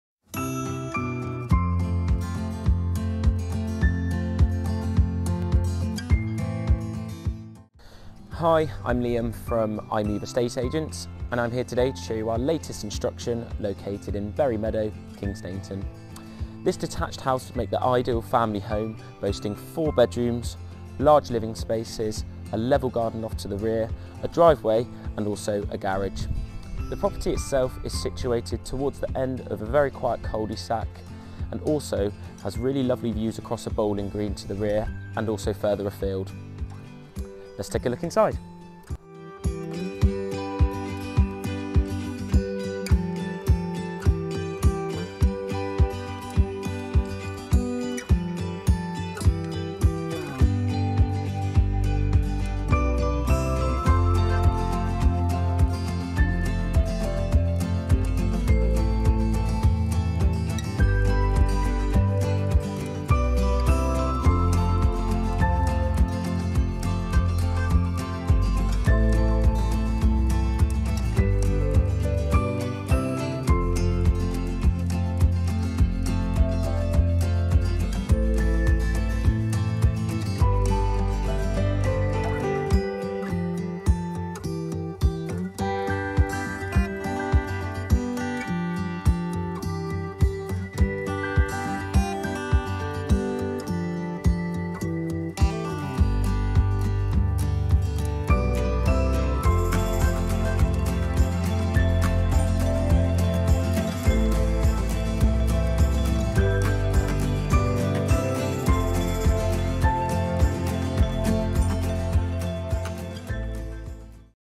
Berry Meadow, TQ12
An opportunity to purchase a property with great potential to make a fantastic family home, situated towards the end of a quiet cul-de-sac, with beautiful views across a bowling green to the rear. Benefitting from a driveway, garage, large living spaces, level garden, 4 bedrooms plus much more.